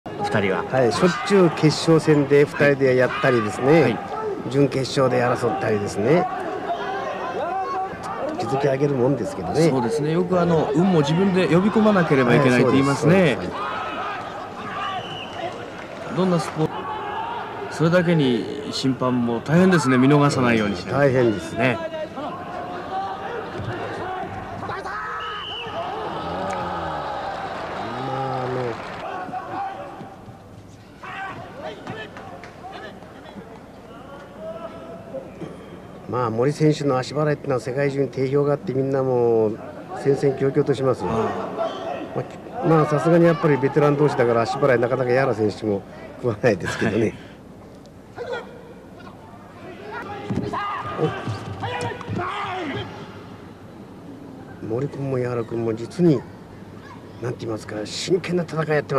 矢原 美紀夫 vs 森 俊博
第25回全国空手道選手権大会（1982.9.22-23／日本武道館)
男子組手準決勝　矢原 美紀夫 vs 森 俊博

1982年9月22日から23日にかけて、日本武道館で開催された
第25回全国空手道選手権大会の男子組手準決勝で、矢原美紀夫選手と森俊博選手が対戦しました。